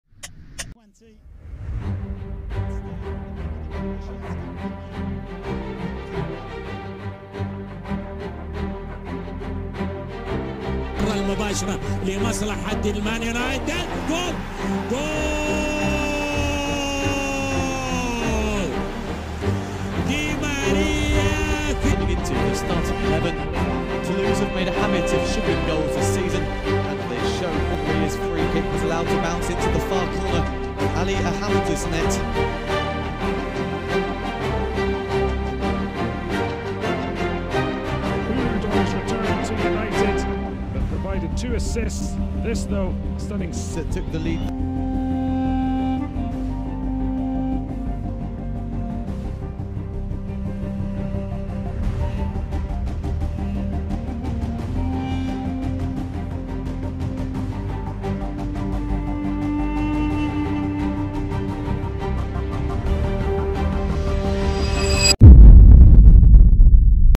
Di María ONLY scores Free-Kicks on Accident…
Ángel Di María from Argentina, Paris Saint-Germain F.C. and Juventus F.C. really only scoring Accidental Free-Kick goals